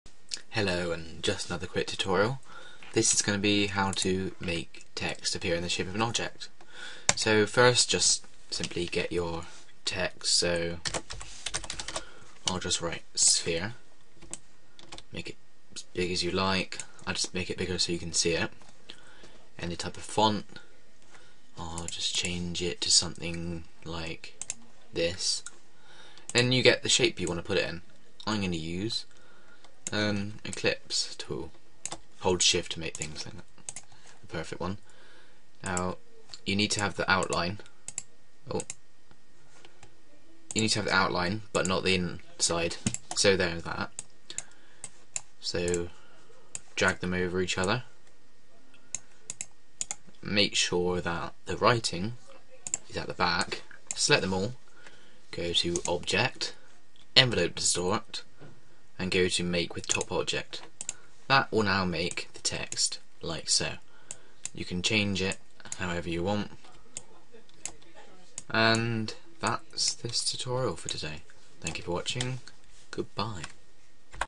Make text a certain shape illustrator (envelope distort)
a tutorial on how to make your text go into a certain shape